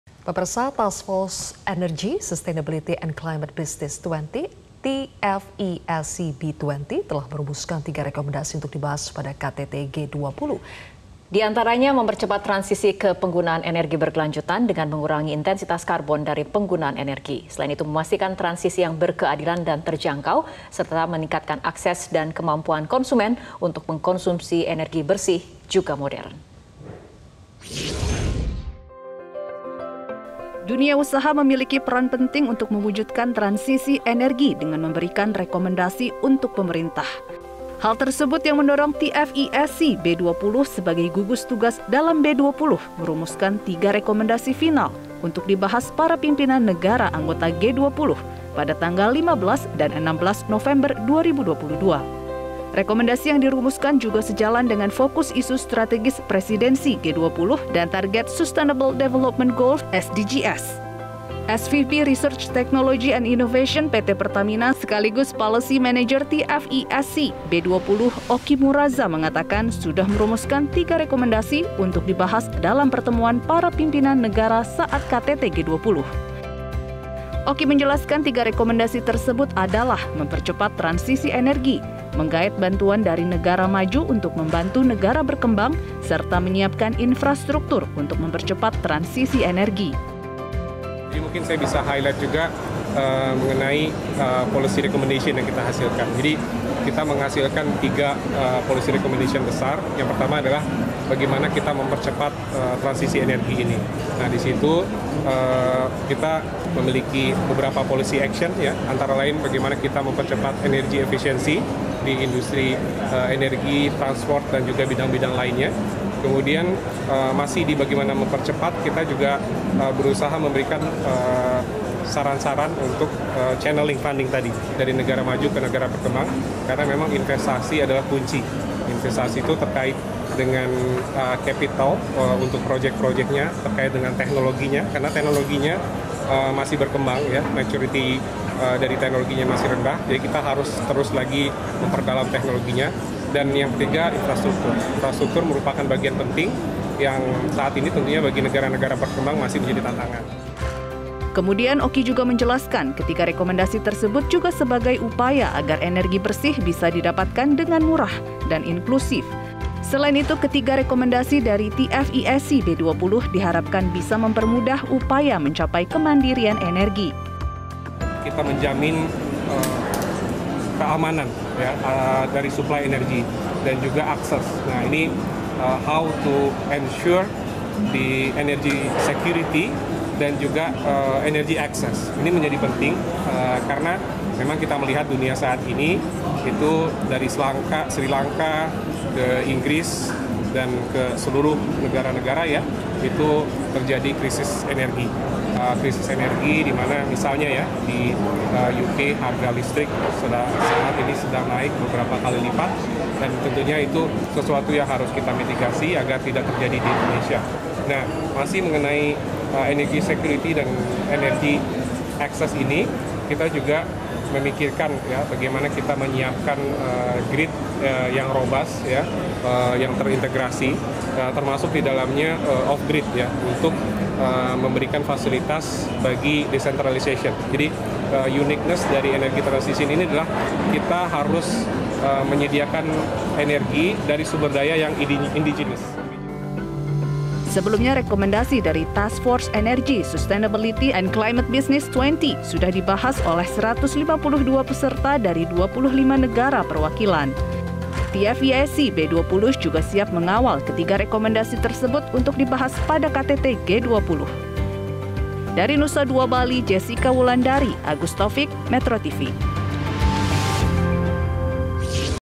Tiga Rekomendasi Kebijakan TF ESC B20 di KTT G20
MetroTV, Task Force Energy, Sustainability and Climate Business 20 (TF ESC-B20) telah merumuskan tiga rekomendasi untuk di bahas di KTT G20. Diantaranya mempercepat transisi energi berkelanjutan dengan mengurangi intensitas karbon dari penggunaan energi.

Selain itu memastikan transisi yang berkeadilan dan terjangkau setelah meningkatkan akses dan kemampuan konsumen untuk mengkonsumsi energi bersih juga modern.